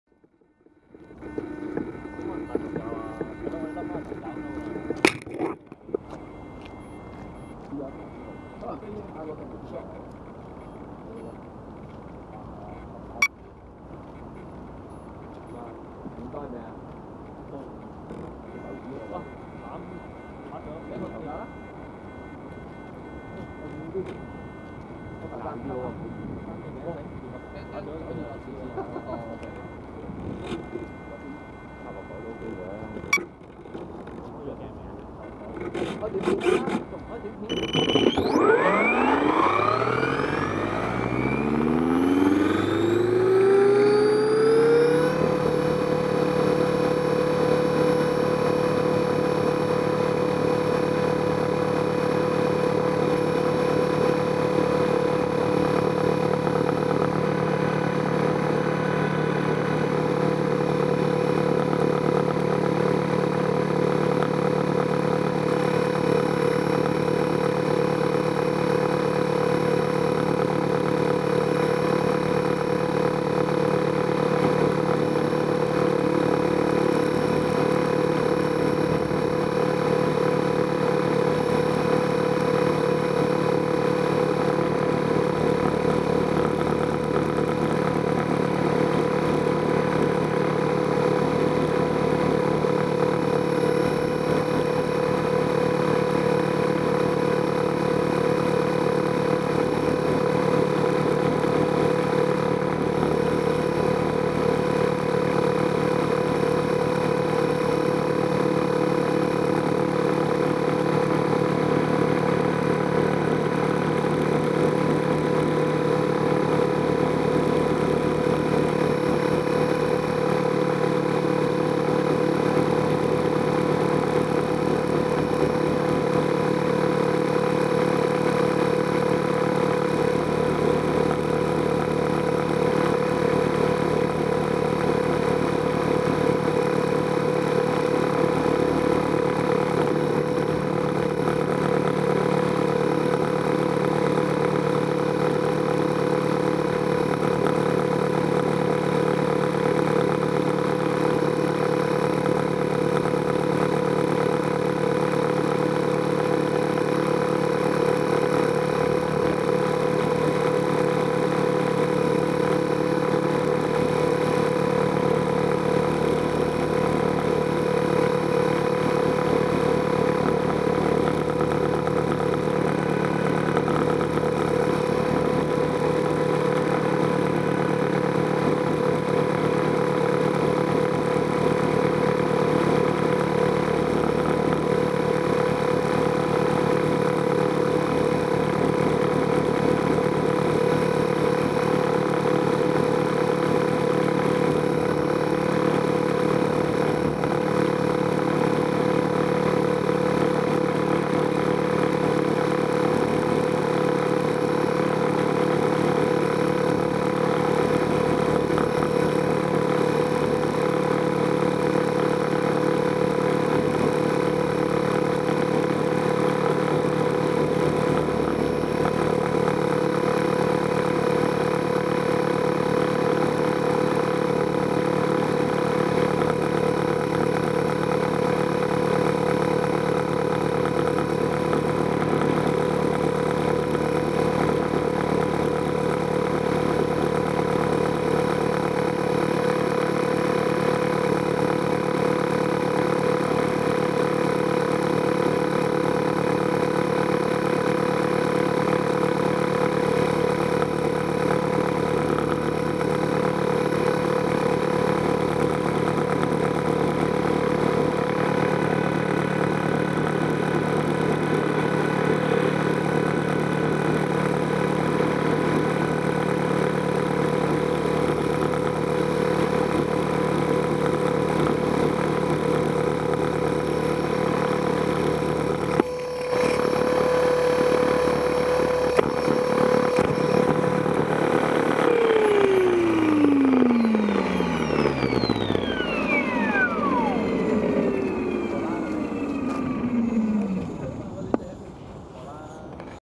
Contour Roam 1080p RC Heli Align-450 SE v2 Test
Mounted on landing skid, vibration at 2200rpm caused distortion.
Jello effect big time.